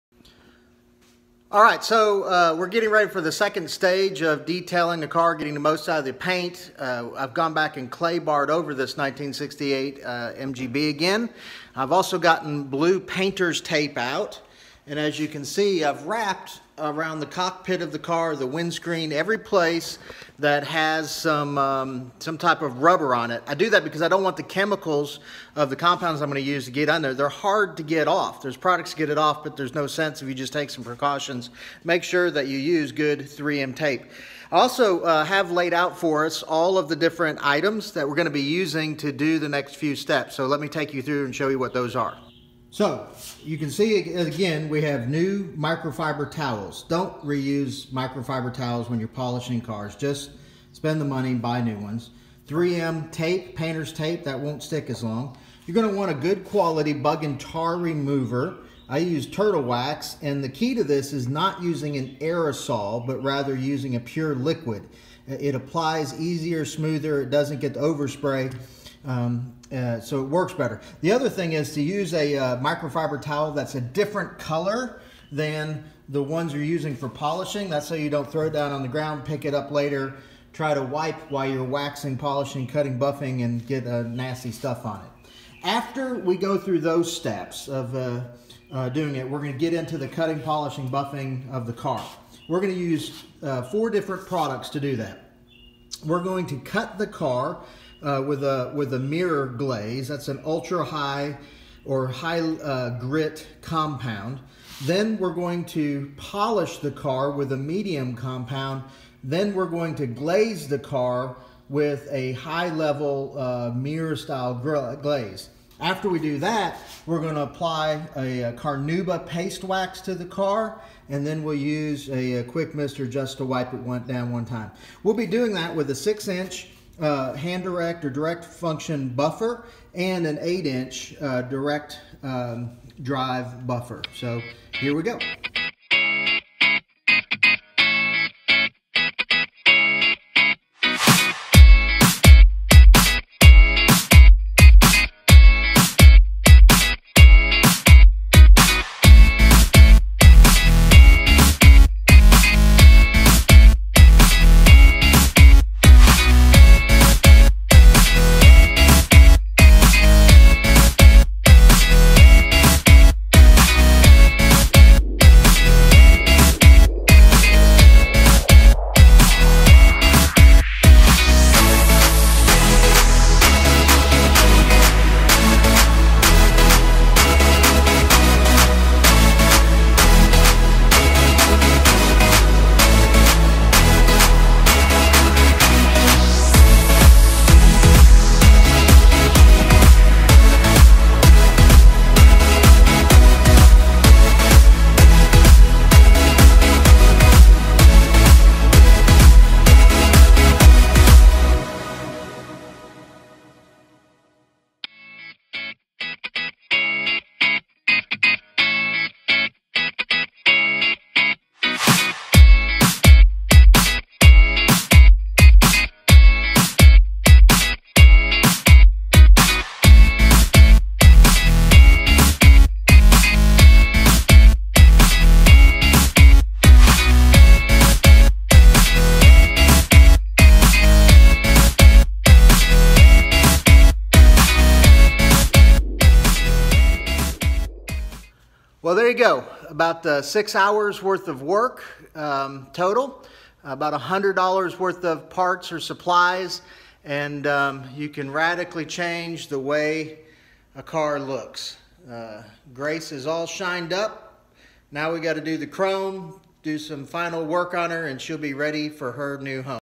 Detail you MG or other unique car: Part 2
Step by step videos showing how to cut, polish, glaze and wax your car.  Get the best out of your paint. Bryan Hutchinson, MKII Motorsports, Dish, TX